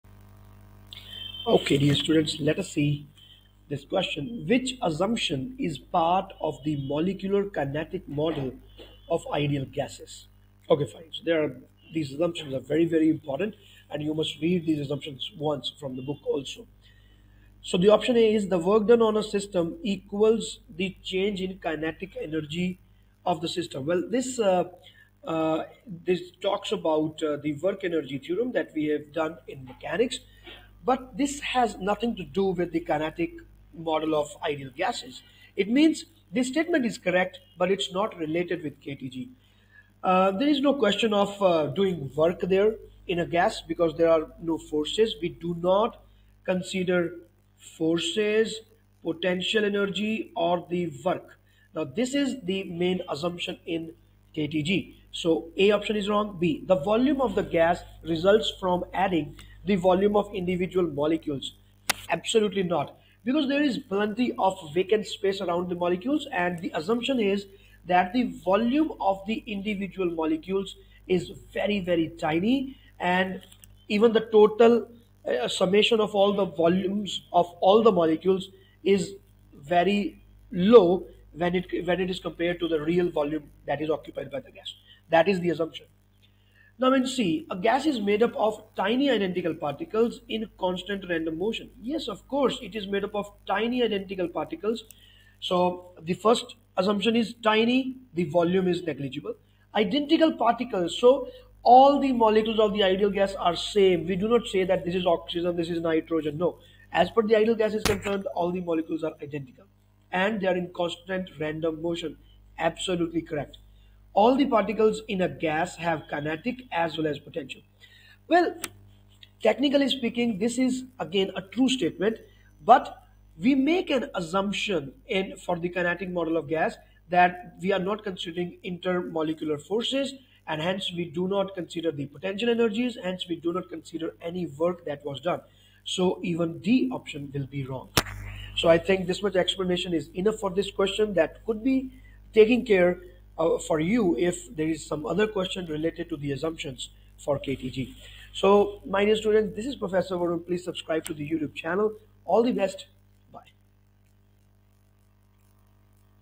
Which assumption is part of the molecular kinetic model of ideal gases?A. The work done on
Which assumption is part of the molecular kinetic model of ideal gases?
A. The work done on a system equals the change in kinetic energy of the system.
B. The volume of a gas results from adding the volume of the individual molecules.
C. A gas is made up of tiny identical particles in constant random motion.
D. All particles in a gas have kinetic and potential energy. Links:
Discord Server for IB Physics by Prof.Varun: